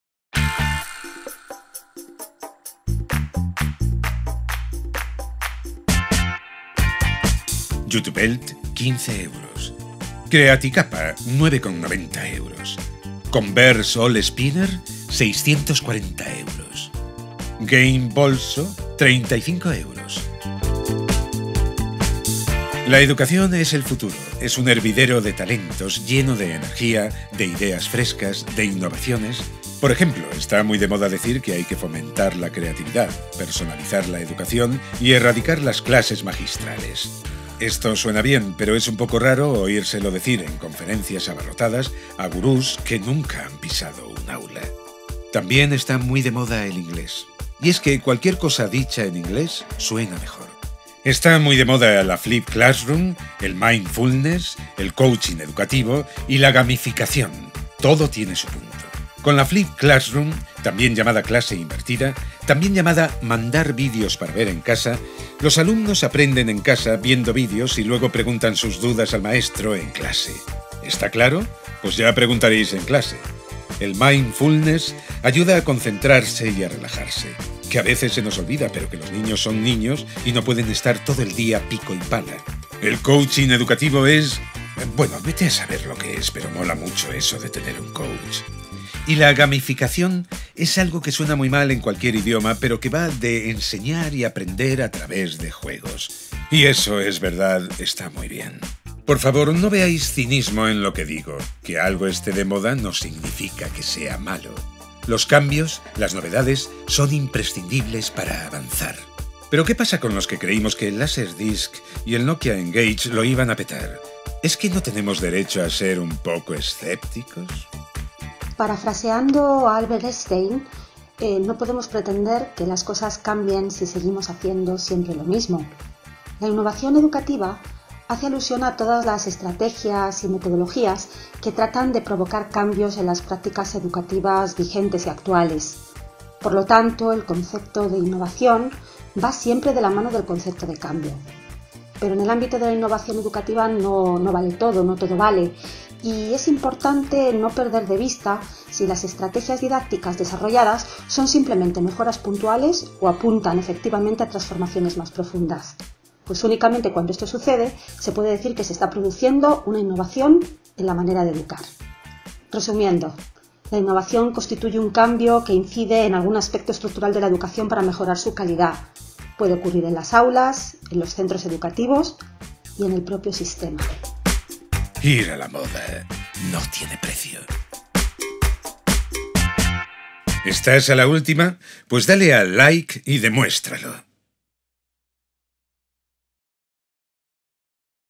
Los docentes visten de Prada (con Estrella López Aguilar)
En el octavo episodio de QuéEducación, Bea se convierte en la protagonista de un anuncio de moda y la maestra Estrella López Aguilar nos explica qué significa innovar en educación.

Temas: Innovación educativa, flipped classroom, mindfulness, bilingüismo, gamificación, coaching, cambio educativo...